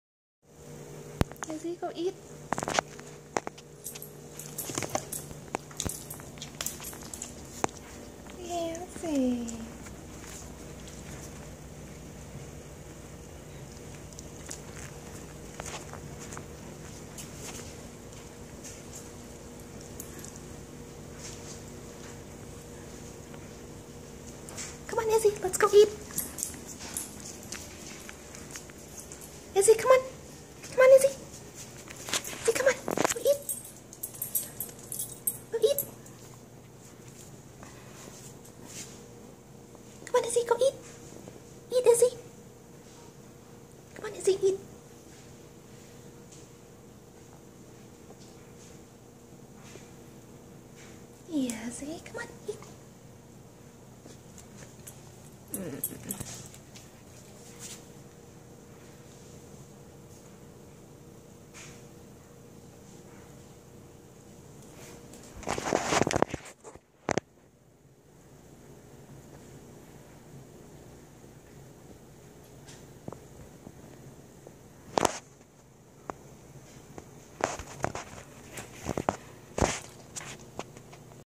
Siva abusing her dog - part 2
Poor Izzy. He had to live on the road for the past month. Two weeks ago he got bit and developed an abcess requiring shaved fur patch and antibiotics. He got bullied and toys stolen by his uncle Terry. Two days ago he injured his paw pad. Today he got a bath and his foot wrapped up. :( Poor Izzy